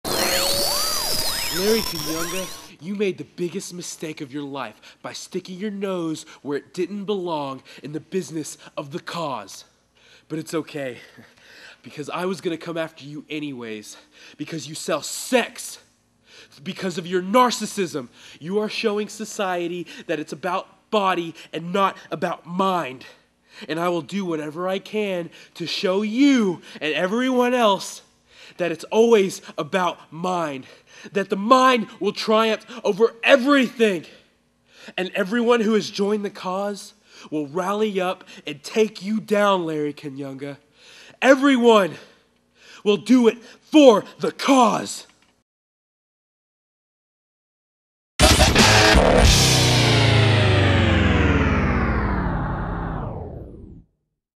Joey The Cos! Addresses Larry Canyunga
Joey the Cos! Addresses Larry Canyunga for his intervention during a confrontation with Gregory Symonds. The Cos! seems to feel like Larry has bit off more than he can chew. See What happens on April 25th 2009 in Decatur, TX  at Ruben's Ballroom at 1982 E Highway 380 Decatur, TX as they go one on one! Or as Larry would say: "mano e mano"  (spelling)?